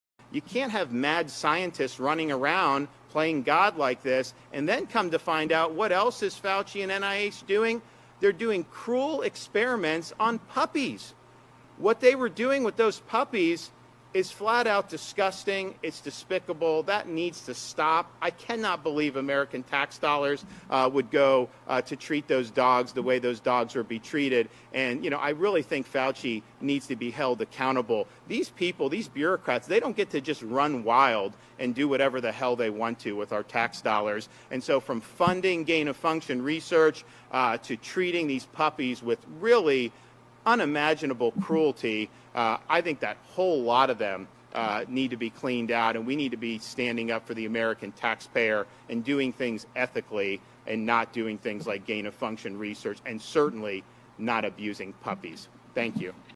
"Fauci needs to be held accountable" Ron DeSantis about Fauci experiments on puppies
Florida Governor Ron DeSantis:” You can’t have mad scientists running around playing God like this…they’re doing cruel experiments on puppies …I really think Fauci needs to be held accountable…”